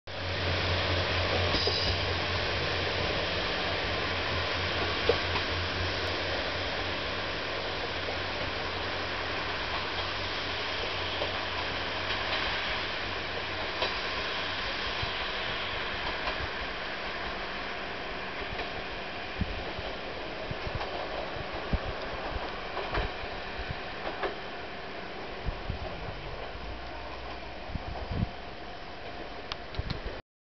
JR東日本 キハ40系気動車@津軽二股駅
JR東日本(東日本旅客鉄道) キハ40系気動車@津軽線 津軽二股駅(青森県東津軽郡今別町大字大川平字清川87-2)

キヤノン パワーショット A470 で撮影

KiHa 40 series DMU, East Japan Railway Company (JR East) at Tsugaru-Futamata Station on Tsugaru Line, Ōkawadai, Imabetsu, Higashitsugaru District, Aomori Prefecture, Japan

Recorded by Canon PowerShot A470